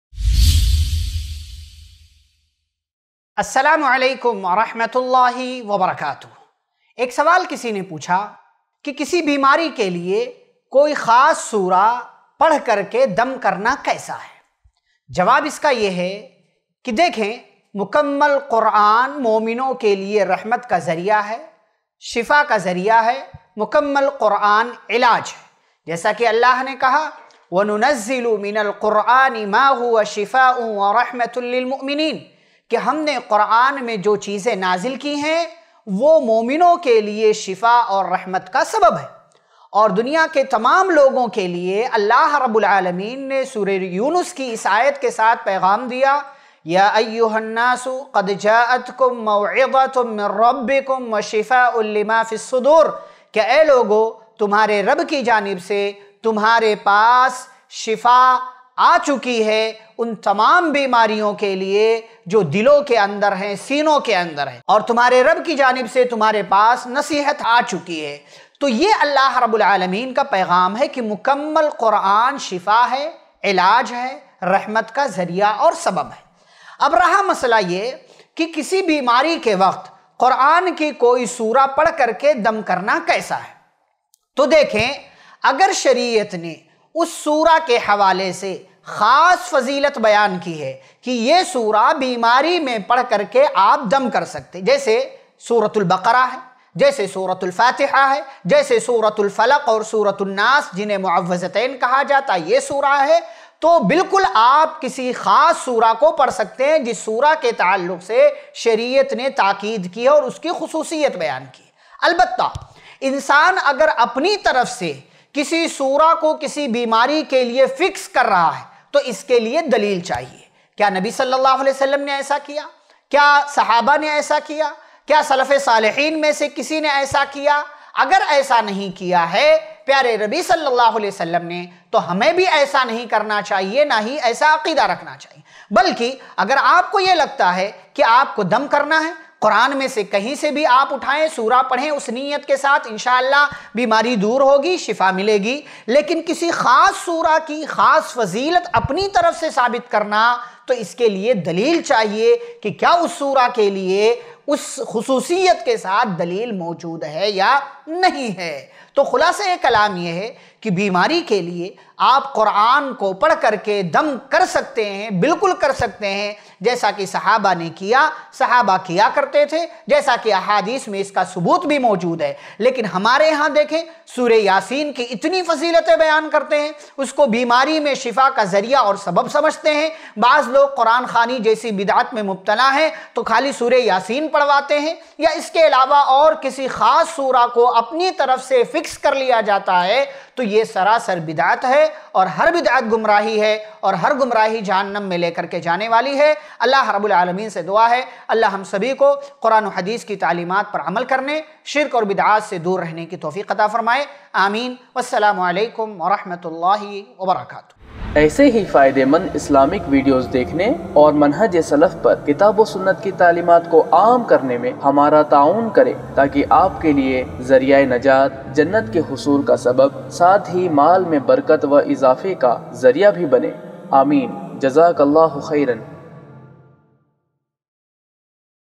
Bimari Ke Liye Koi Surah Padh Kar Dum Karna Kaisa Hai? Sawal O Jawab Series Ep.3 | IIC Mumbai
Bank Account Details:
ILM FOUNDATION
ICICI Bank
Savings account
IFSC CODE-ICIC0001028

Bimari Ke Liye Koi Surah Padh Kar Dum Karna Kaisa Hai? Sawal O Jawab Series Ep.3 | Hafiz Khalil Ur Rahman Sanabili | IIC Islam Helpline